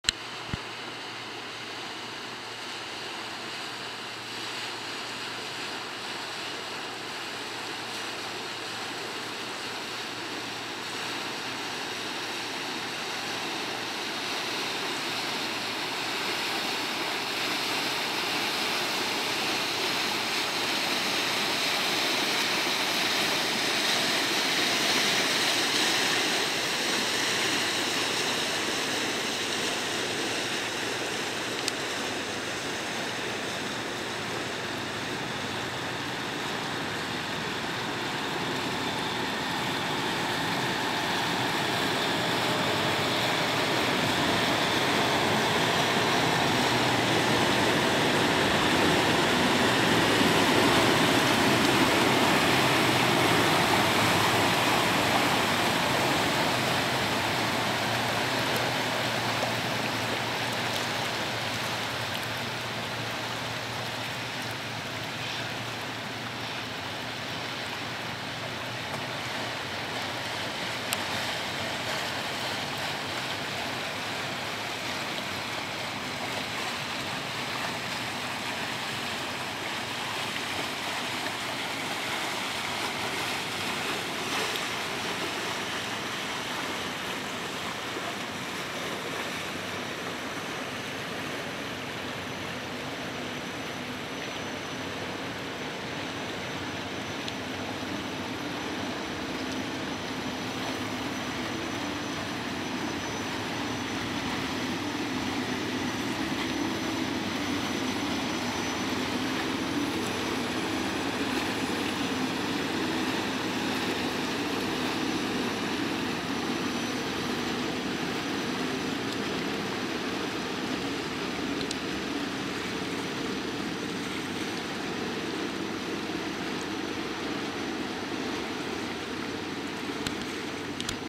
Orientis begegnet Micanto.
Direkt vor der Slipanlage Abbesbüttel.
Orientis fährt nach Osten.